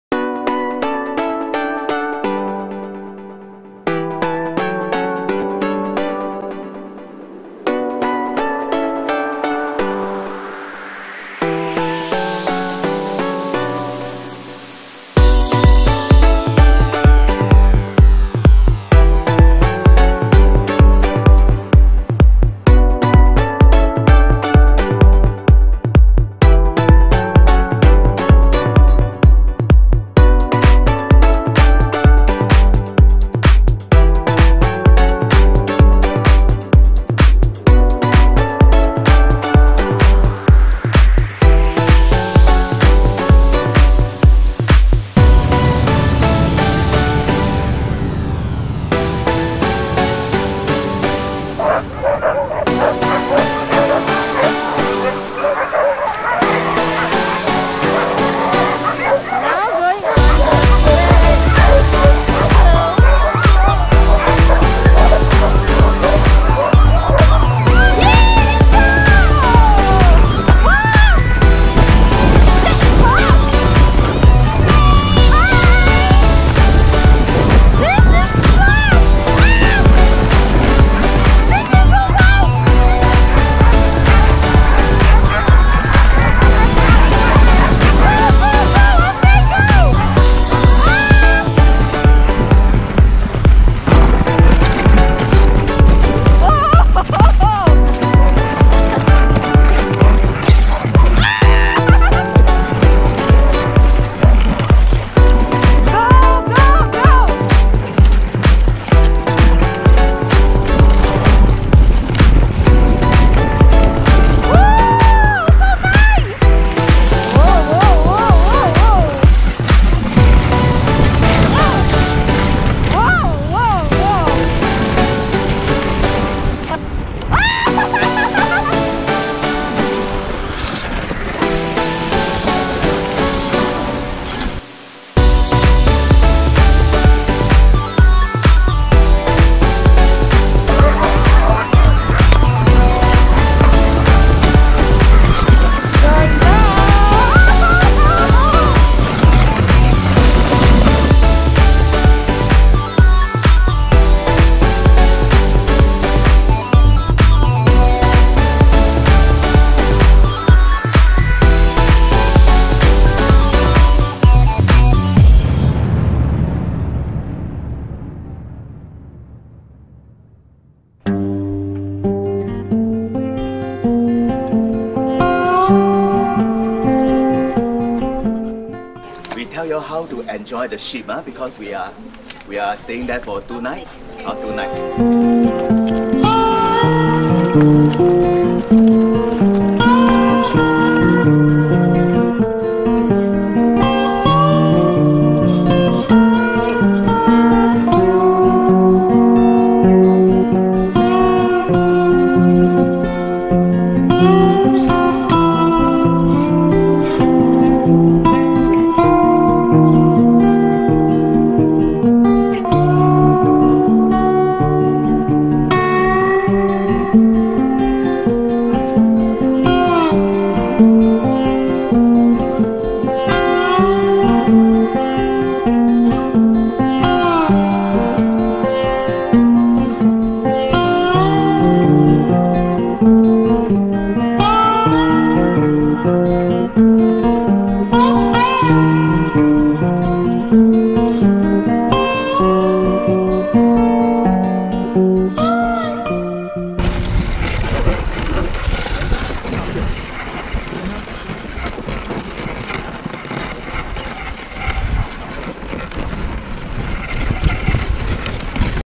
Day 4 - Dog Sledge and Drone flying at -20 degrees temp
Dog sledge riding with drone view at Norway.

Equipment:
GoPro with Snoppa Stabilizer
DJI Mavic Pro
Xiaomi Mi Mix
Panasonice Lumix GX7